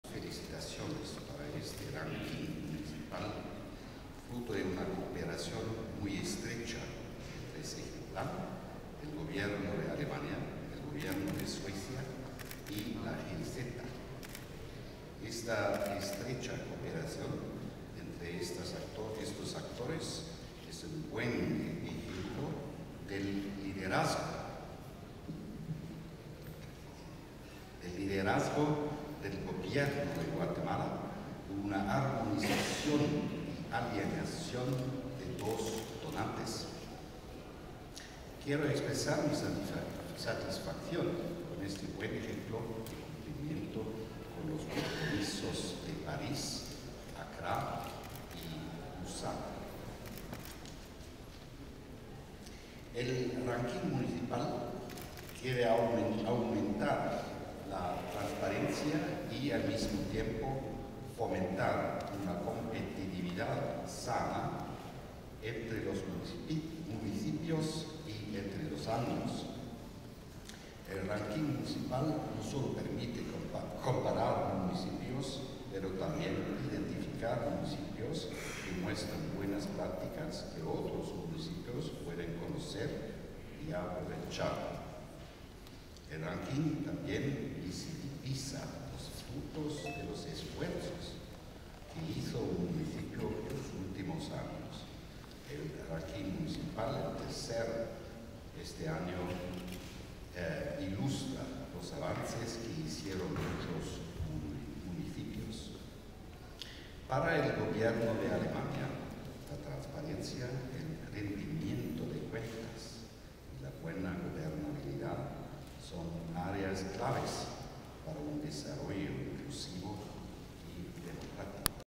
Palabras Matias Peter Sonn, Embajador Alemania
Patzún, Palabras Matias Peter Sonn, Embajador Alemania en la presentación del Ranking de gestión Municipal ejecutado por SEGEPLAN y GIZ